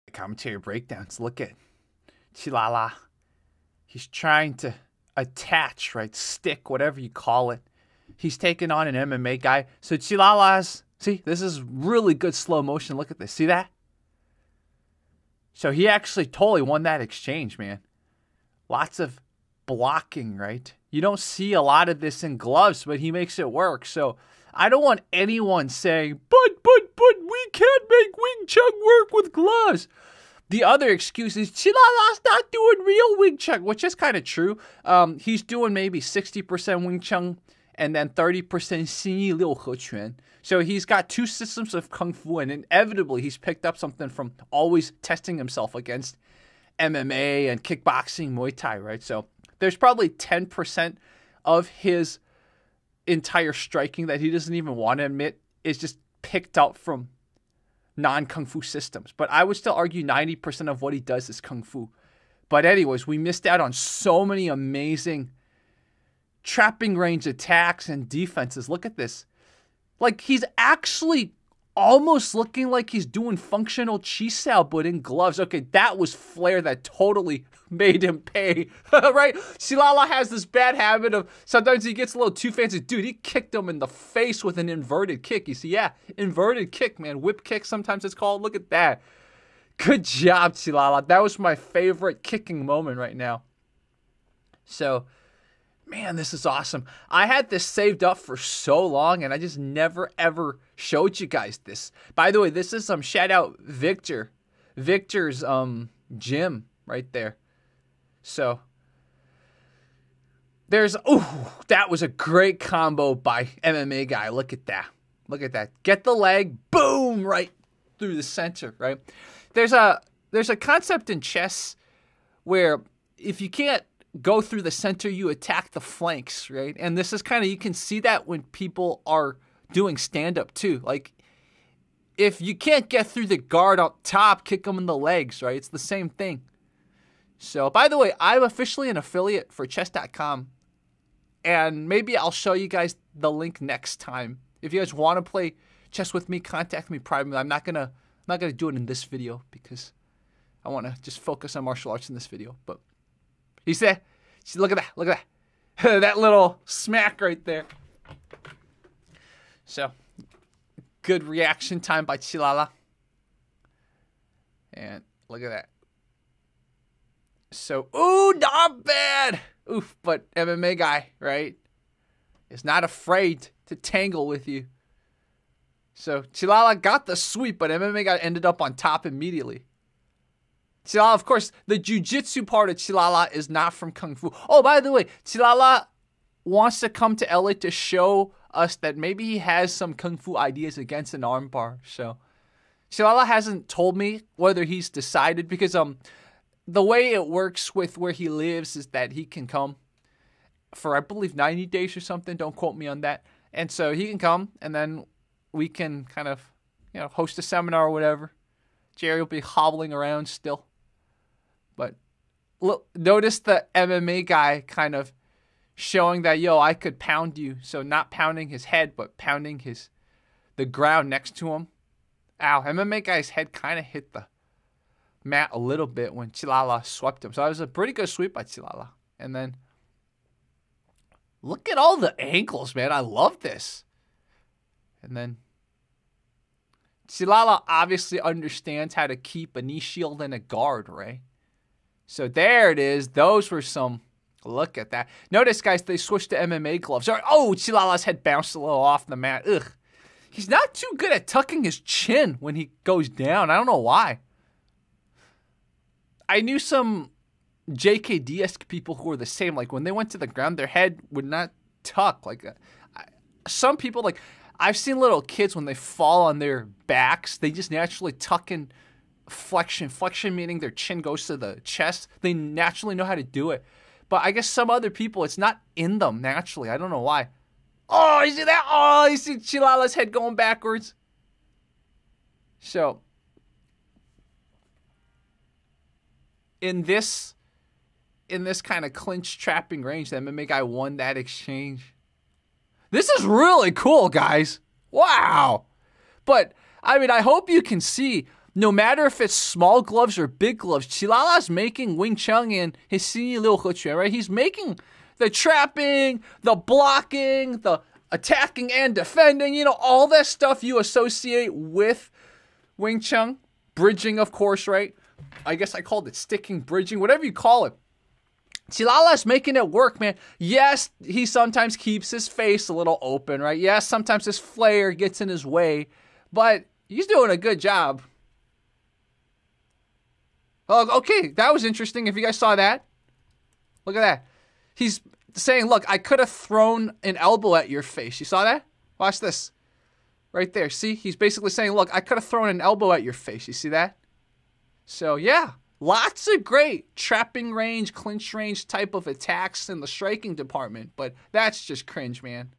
When You Pull Off Awesome Kungfu Against MMA - Qi La La Spars Johnny Chang
Qi La La tests his understanding kungfu concepts again testing trapping, bridging, blocking, etc. against an mma guy named Johnny Chang. Let's watch and react to this awesome sparring! Let us know what you saw!